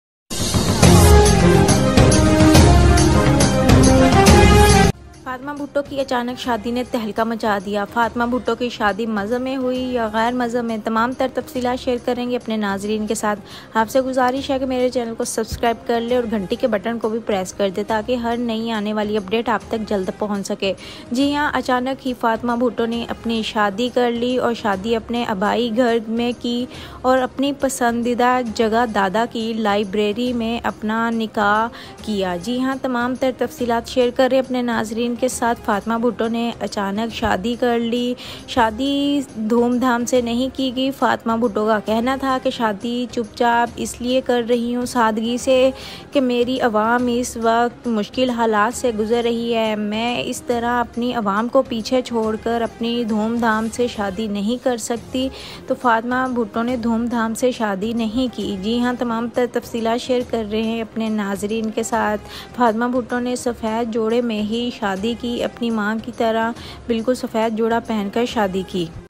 Fatima Bhutto marriage with London famous actor | fatima Bhutto new pictures viral | Bhutto family
Fatima Bhutto marriage with hamza shabaz | fatima Bhutto new pictures viral | Bhutto family